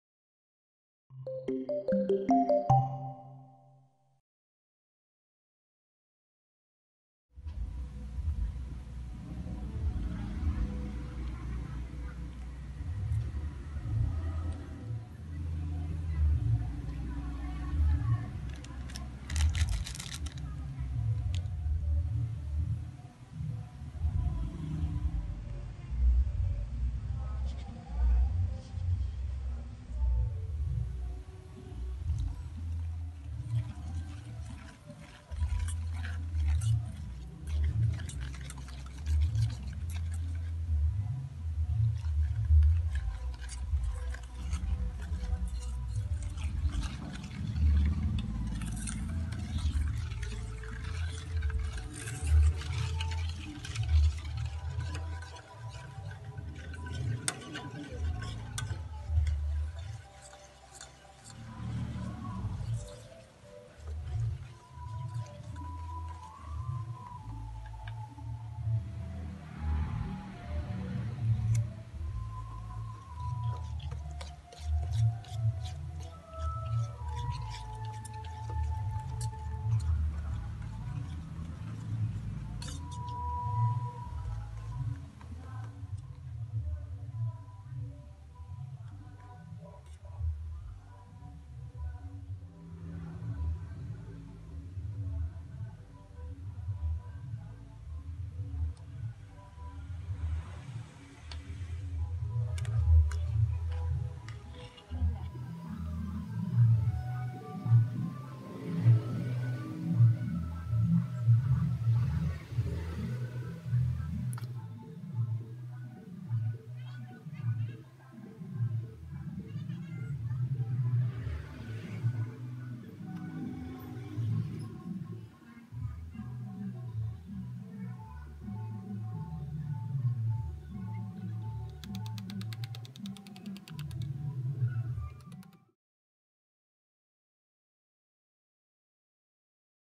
RESEP SELAI COKLAT HOMEMADE UNTUK ROTI TAWAR
Inilah Rahasia Resep selai coklat home made untuk dimakan dengan roti tawar.

Cara membuat selai coklat ini pun sangat mudah, jadi bisa dibuat sebagai cemilan atau dengan roti tawar yang cocok dimakan bareng keluarga.

Yuk tanpa basa basi lagi, langsung saja simak resep selai coklat homemade sebagai cemilan dan topping favorit anak-anak kita berikut ini.

Terimakasih sudah nonton,sampai jumpa di video selanjutnya